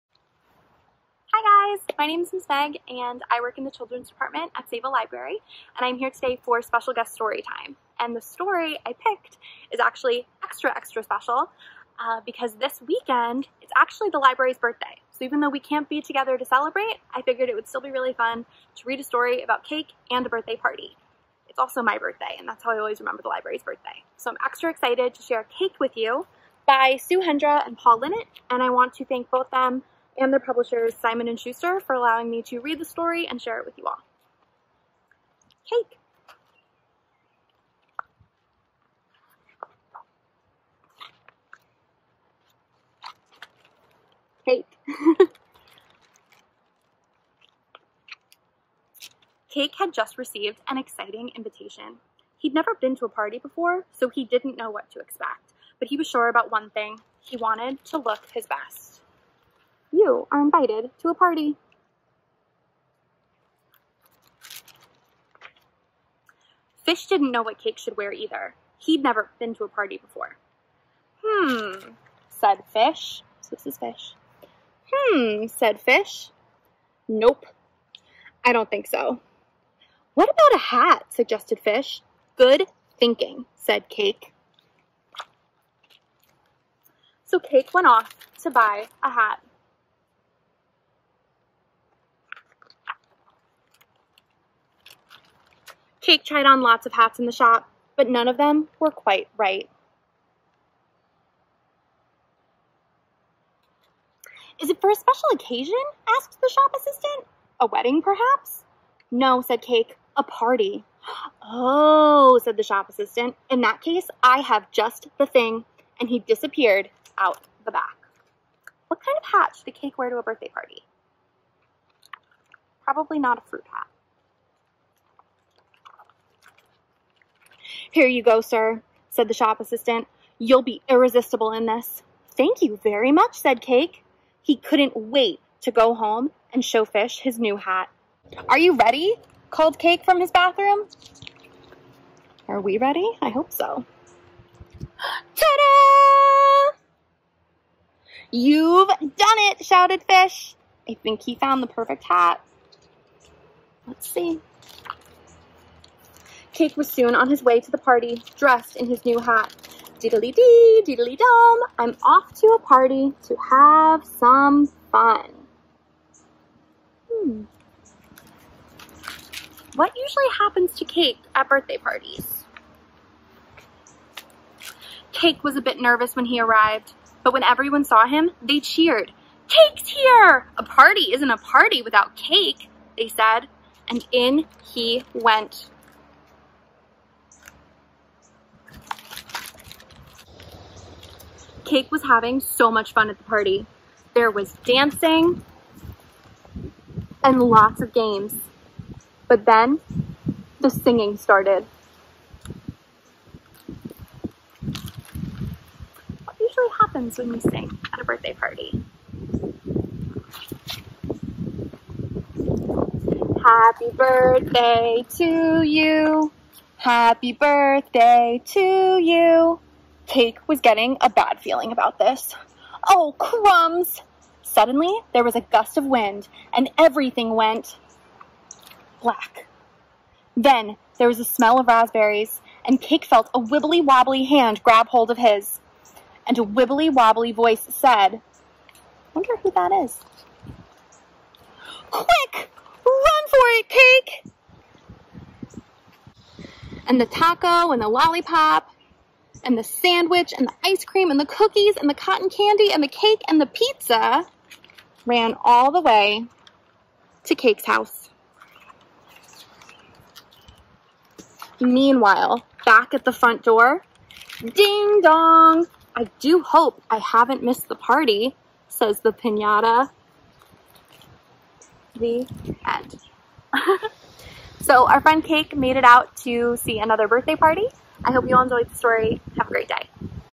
Storytime Corner - Cake! - Miss Meg
Miss Meg is our Special Guest today for a birthday story. Sunday is Sayville Library's 11th anniversary of being in our building on Greene Ave and it's also Miss Meg's birthday! She's more than 11 years though!